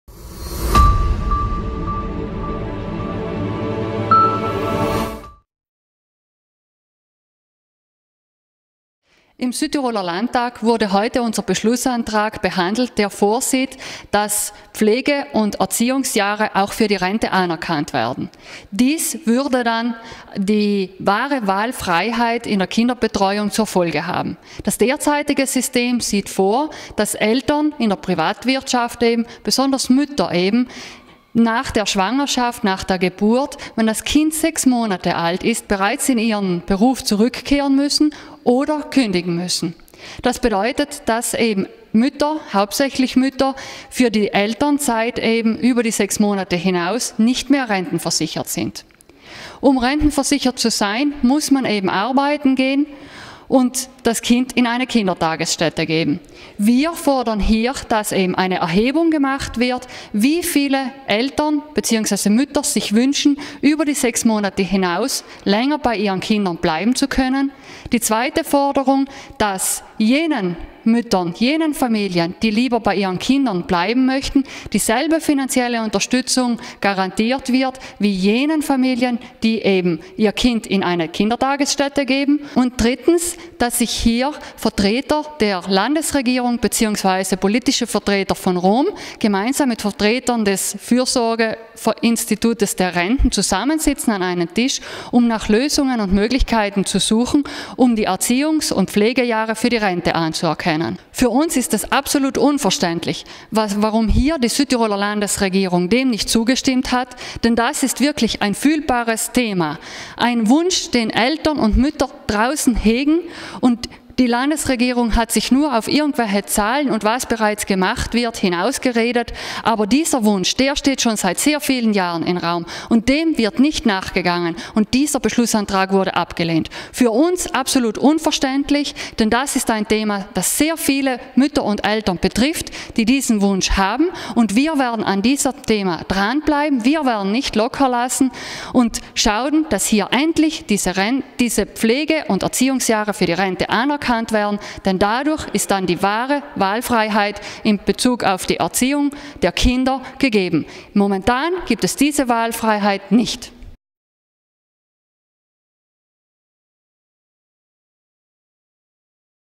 Landesregierung sagt Nein zur Rente für Kinderbetreuung und Pflege
Die Landtagsabgeordnete der Süd-Tiroler Freiheit, Myriam Atz Tammerle, zeigt sich entrüstet, dass sich die Landesregierung nicht für die Anerkennung der Pflege- und Kinderbetreuungszeit für die Rente einsetzt. Im Filmbeitrag erklärt sie die Hintergründe des Antrags und die unverständliche Reaktion der Landesregierung.